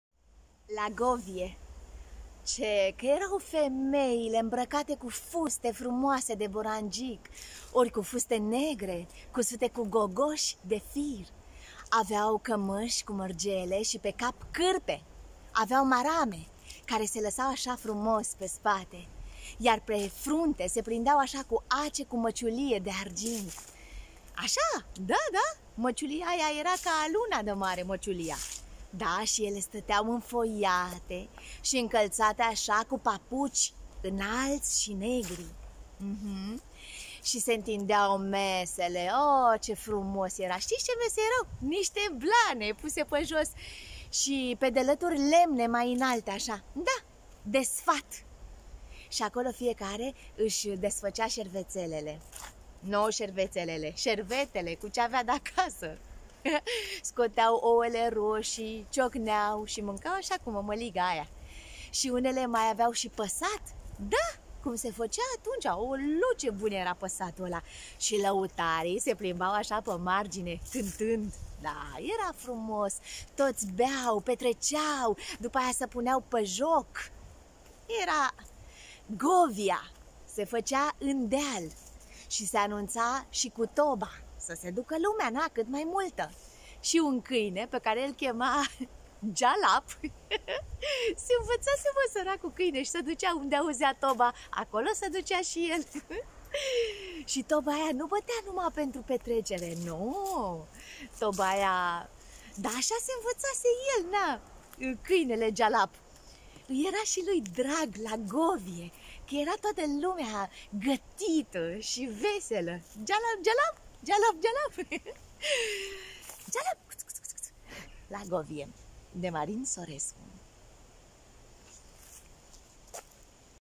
La Govie de Marin Sorescu
Bună dimineața 7 noiembrie 2020, in pădure, sâmbătă, toamna
Govia înțeleasă ca petrecerea la țară, unde se intalneau familii, oamenii satului gătiți frumos, in straie populare care mai de care și întindeau eau ștergare si mâncau si petreceau împreună, de vreo sărbătoare...(sau vreo pomenire, dar nu se mai numea govie atunci. )
Câinele Gealap e personajul principal se pare in mica povestire, cățel care era abonat si cerșetor pe la toate ,, mesele,,
Vă amintiți de vreo petrcere din asta? Sau un cățel iubit cerșetor, de care nu puteai scăpa, itinintra si suflet si brațe?
Va îmbrățișez cu dragoste mare.